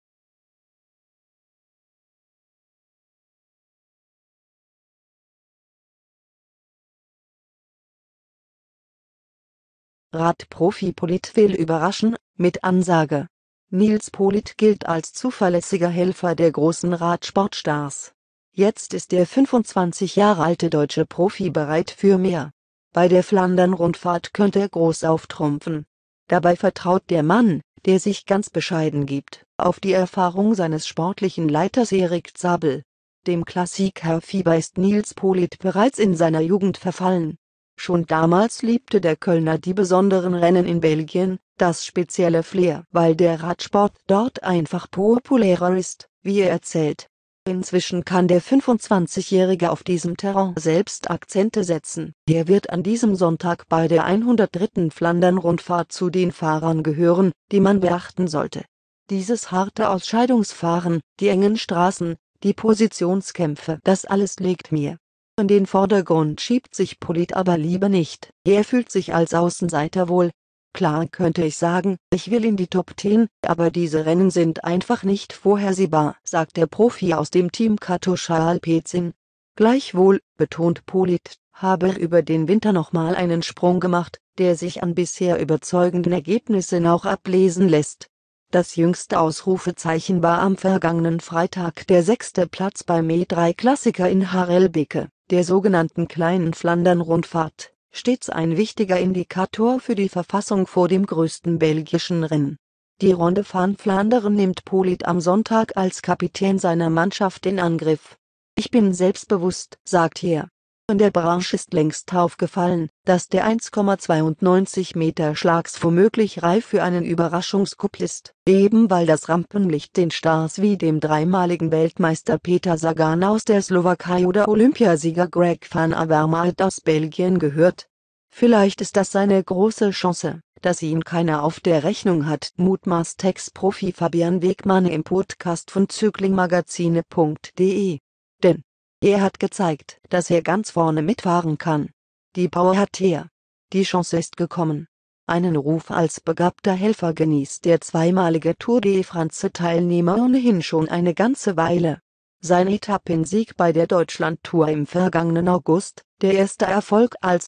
Radprofi Politt will überraschen - mit Ansage
Nils Politt gilt als zuverlässiger Helfer der großen Radsport-Stars. Jetzt ist der 25 Jahre alte deutsche Profi bereit für mehr: Bei der Flandern-Rundfahrt könnte er groß auftrumpfen. Dabei vertraut der Mann, der sich ganz bescheiden gibt, auf die Erfahrung seines sportlichen Leiters Erik Zabel.

Dem Klassikerfieber ist Nils Politt bereits in seiner Jugend verfallen. Schon damals liebte der Kölner die besonderen Rennen in Belgien, das spezielle Flair, "weil der Radsport dort einfach populärer ist", wie er erzählt. Inzwischen kann der 25-Jährige auf diesem Terrain selbst Akzente setzen, er wird an diesem Sonntag bei der 103. Flandern-Rundfahrt zu den Fahrern gehören, die man beachten sollte: "Dieses harte Ausscheidungsfahren, die engen Straßen, die Positionskämpfe, das alles liegt mir."

In den Vordergrund schiebt sich Politt aber lieber nicht, er fühlt sich als Außenseiter wohl. "Klar könnte ich sagen, ich will in die Top Ten, aber diese Rennen sind einfach nicht vorhersehbar", sagt der Profi aus dem Team Katusha-Alpecin. Gleichwohl, betont Politt, habe er "über den Winter nochmal einen Sprung gemacht", der sich an bisher überzeugenden Ergebnissen auch ablesen lässt. Das jüngste Ausrufezeichen war am vergangenen Freitag der sechste Platz beim E3-Klassiker in Harelbeke, der sogenannten kleinen Flandern-Rundfahrt, stets ein wichtiger Indikator für die Verfassung vor dem größten belgischen Rennen.

Die "Ronde van Vlaanderen" nimmt Politt am Sonntag als Kapitän seiner Mannschaft in Angriff. "Ich bin selbstbewusst", sagt er. In der Branche ist längst aufgefallen, dass der 1,92-Meter-Schlaks womöglich reif für einen Überraschungscoup ist, eben weil das Rampenlicht den Stars wie dem dreimaligen Weltmeister Peter Sagan aus der Slowakei oder Olympiasieger Greg van Avermaet aus Belgien gehört. "Vielleicht ist das seine große Chance, dass ihn keiner auf der Rechnung hat", mutmaßt Ex-Profi Fabian Wegmann im Podcast von cyclingmagazine.de. Denn: "Er hat gezeigt, dass er ganz vorne mitfahren kann. Die Power hat er."

Die Chance ist gekommen.

Einen Ruf als begabter Helfer genießt der zweimalige Tour-de-France-Teilnehmer ohnehin schon eine ganze Weile. Sein Etappensieg bei der Deutschland Tour im vergangenen August, der erste Erfolg als Radprofi, war aber ein Schlüsselerlebnis. Politt wurde bewusst: Ich kann selbst Rennen gewinnen. "Die Hoffnung wird größer, die auf ihm ruht", sagt Wegmann. Die Reife für die großen Klassiker holte sich Politt in den vergangenen Jahren im Schatten erfahrener Teamkollegen wie dem Norweger Alexander Kristoff oder Ex-Zeitfahrweltmeister Tony Martin, die beide mittlerweile in anderen Mannschaften fahren. "Da", sagt Politt, "konnte ich mich in der zweiten Reihe langsam entwickeln."

Nun ist seine Chance gekommen. Die Lehrzeit des Allrounders  ist vorüber. Politt besitzt den Biss, die Härte und Schmerztoleranz für die giftigen Hellingen in Flandern oder auch das gefürchtete Kopfsteinpflaster von Paris-Roubaix, das ihm "noch einen Zacken" besser liegt. In Dirk Demol und Erik Zabel hat er zudem sportliche Leiter, die ob ihrer Vergangenheit in der Hochdoping-Ära zwar ein ramponiertes Image haben, aber auch einen renntaktisch reichen Erfahrungsschatz besitzen. "Bei Erik sollte man auf jeden Fall genau zuhören", sagt Politt, "was er vorhersagt, tritt meistens auch ein."